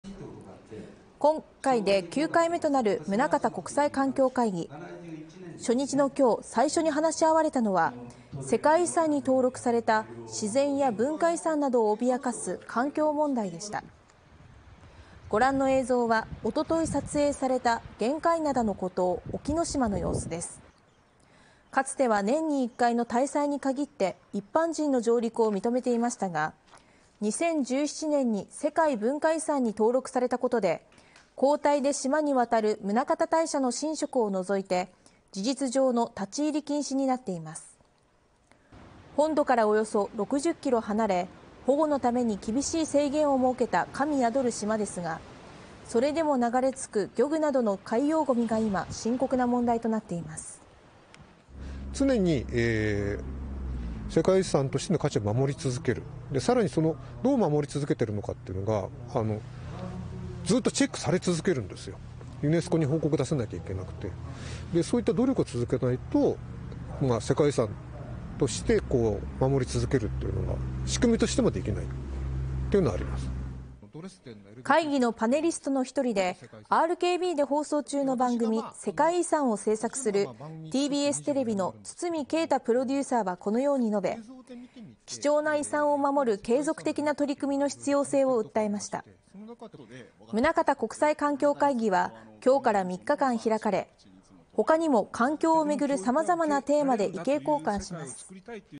玄界灘の孤島「沖ノ島」世界文化遺産の価値守れるか 「宗像国際環境会議」始まる
海洋ごみや海水温の上昇など、さまざまな環境の異変について意見を交わす国際環境会議が、福岡県宗像市で始まりました。話し合われたテーマの一つは、「世界遺産と環境問題」。５年前に世界文化遺産に登録された「神宿る島」、沖ノ島でもごみの問題が起きるなど、貴重な遺産をどう守っていくかが問われています。

◆玄界灘の孤島「沖ノ島」
今回で９回目となる宗像国際環境会議。初日の２６日、最初に話し合われたのは、世界遺産に登録された自然や文化遺産などを脅かす環境問題でした。２４日に撮影された玄界灘の孤島、沖ノ島の様子です。かつては年に１回の大祭に限って一般人の上陸を認めていましたが、２０１７年に世界文化遺産に登録されたことで、交代で島に渡る宗像大社の神職を除いて、事実上の立ち入り禁止になっています。本土から約６０キロ離れ、保護のために厳しい制限を設けた「神宿る島」ですが、それでも流れ着く漁具などの海洋ごみが今深刻な問題になっています。

ＴＢＳテレビ「世界遺産」　堤慶太プロデューサー「常に世界遺産としての価値を守り続ける。さらに、どう守り続けているのか。ずっとチェックされ続けるんですよ、ユネスコに報告しなくてはいけなくて。そういった努力を続けないと、世界尾遺産として守り続けるのが、仕組みとしてできない」

会議のパネリストの１人で、ＲＫＢで放送中の番組「世界遺産」を制作するＴＢＳテレビの堤慶太プロデューサーは、貴重な遺産を守る継続的な取り組みの必要性を訴えました。宗像国際環境会議は２６日から３日間開かれ、ほかにも環境をめぐるさまざまなテーマで意見交換します。